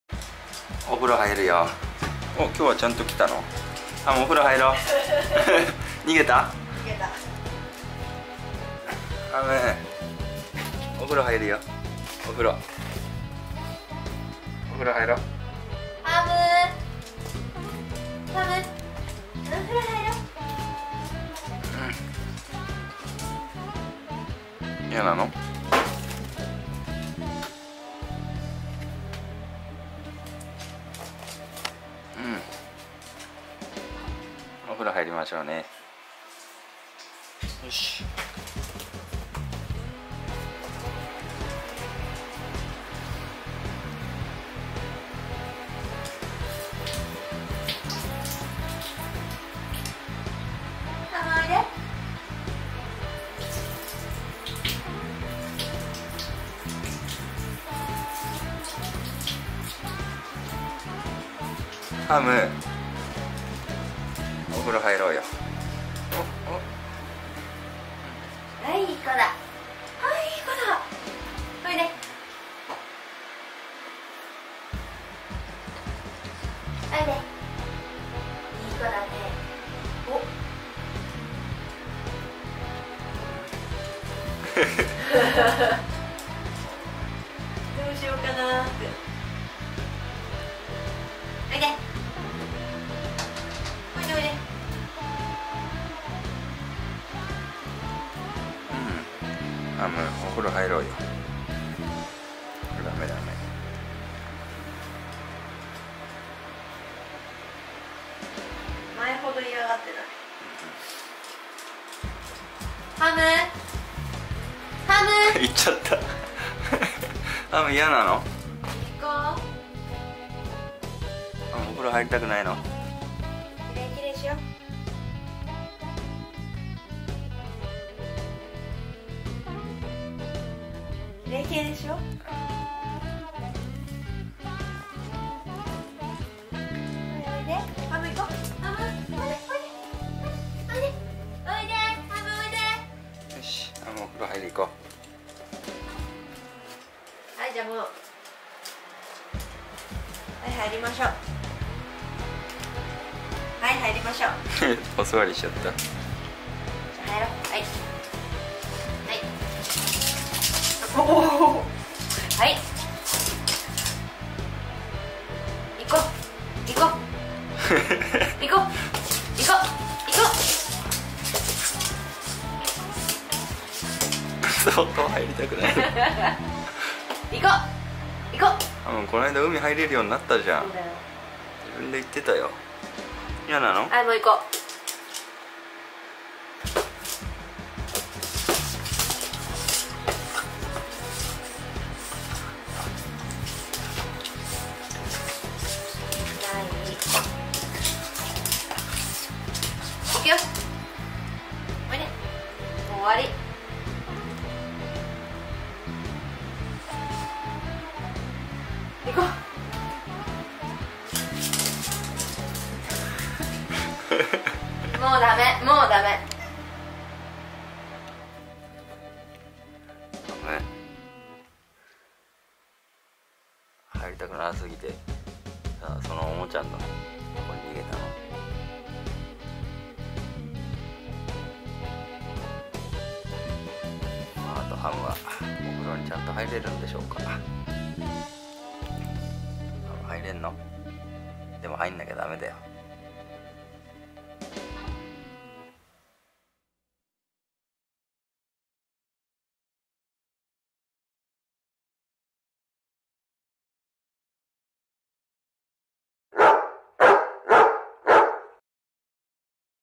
お風呂に入りたくなくてゴネるボストンテリア Boston terrier you do not want to take a bath
ハムのLINEスタンプ作りました🐶❤️😊

お風呂に入りたくなくてゴネるボストンテリア
Boston terrier you do not want to take a bath

使用カメラ Used camera
SONY RX100M6

フレンチブルドッグ じゃないよ|ω･｀) Not a French bulldog

フルーツが大好きなボストンテリアのハムのゆるーい動画です(๑ˇεˇ๑)•*¨*•.¸¸♪